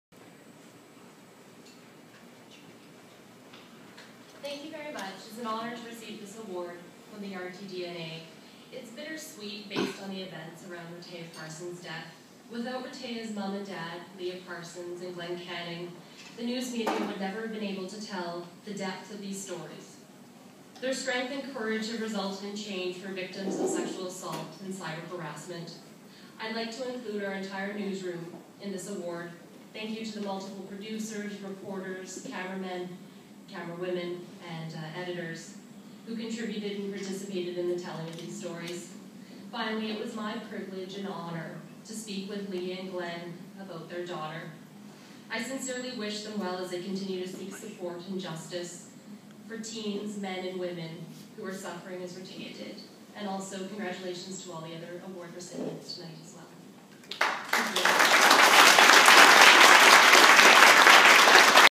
Ron Laidlaw Award for Continuing Coverage
Kelland Sundahl accepts the Ron Laidlaw Award for Continuing Coverage at the 2014 RTDNA Atlantic Regional Awards Banquet